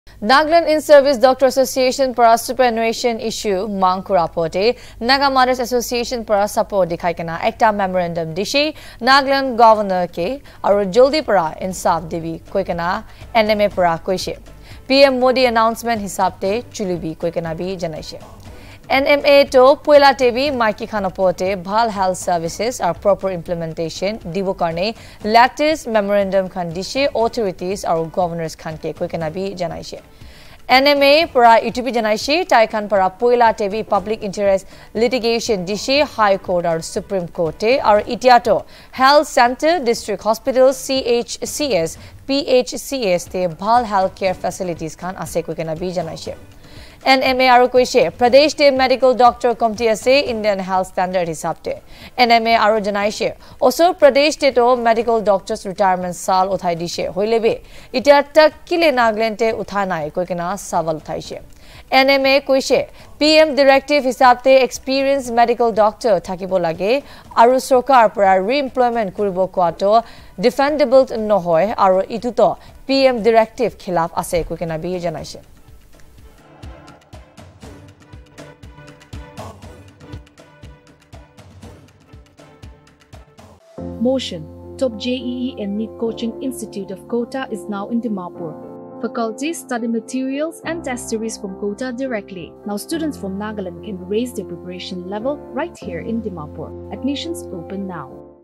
NAGA MOTHERS’ ASSOCIATION EXTENDS ITS SUPPORT TO NIDA’S AGITATION AGAINST NAGALAND GOVT.
The Naga Mothers’ Association (NMA) has extended their support to the Nagaland In-service Doctors’ Association and questioned the decision of the state government to NIDA’s demand. NMA President Abei-Ü Meru and Joint Secretary, Malsawmthangi Leyri in a memorandum to the Nagaland Governor dated July 20, conveyed deep concern about continuing superannuation issue relating to NIDA and sought immediate intervention from the Governor. Pointing out that the increase in retirement age of all medical doctors in the country to 65 years as announced by the Prime Minister in May 2016 was due to an acute shortage of medical doctors in the country, NMA said almost all states including neighbouring north-eastern states have implemented the raise in the age of retirement of medical doctors accordingly. The Mother’s Association also said that through such PILs, many health centres, district hospitals, CHCs and PHCs have better facilities and services today in different parts of the state. However, the acute shortage of medical doctors is still present in the state as per the Indian Health Standard